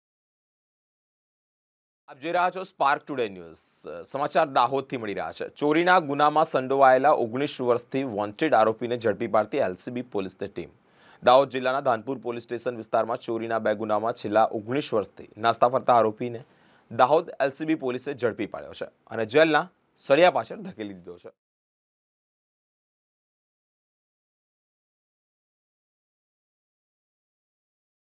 દાહોદમા ચો-રીના ગુનામા સંડોવાયેલ 19 વર્ષથી વોન્ટેડ આરોપીને ઝડપી પાડતી એલસીબી ટીમ 2024 | Vadodara
દાહોદમા ચો-રીના ગુનામા સંડોવાયેલ 19 વર્ષથી વોન્ટેડ આરોપીને ઝડપી પાડતી એલસીબી ટીમ